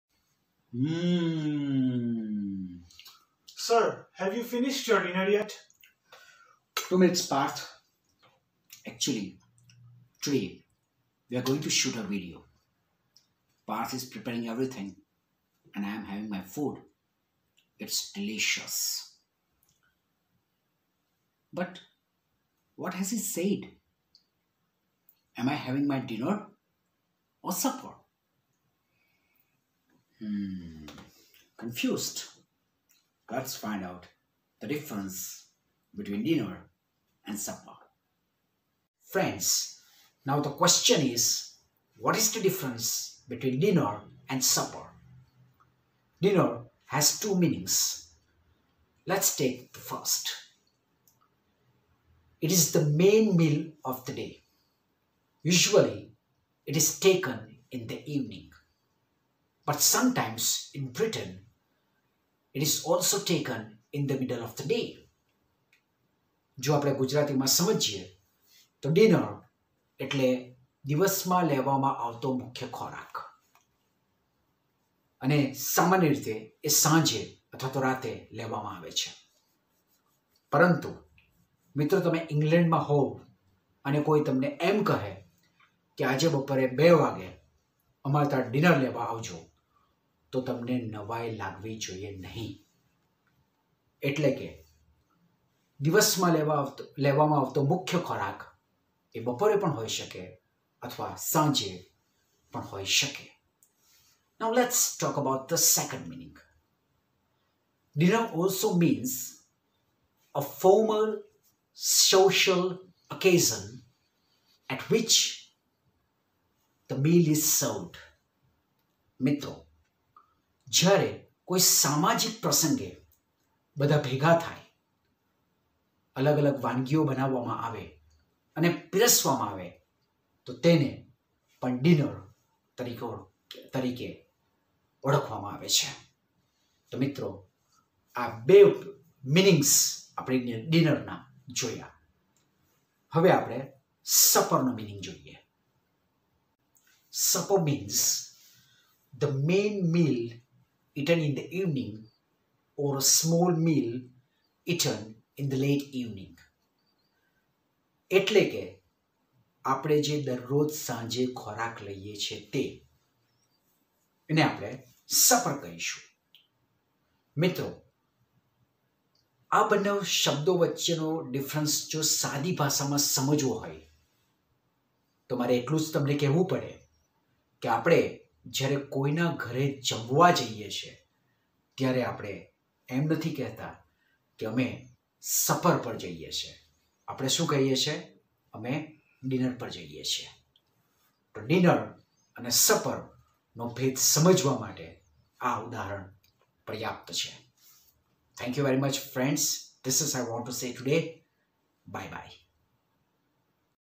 Difference between supper and dinner
What is the difference between supper and dinner?
Can we invite someone for dinner at 2:00 pm?
Do we attend a DINNER party or SUPPER party?

Watch this video to clear up all the doubts.

UNEC - The Centre for English Teaching and Training
Adajan, Surat(Gujarat)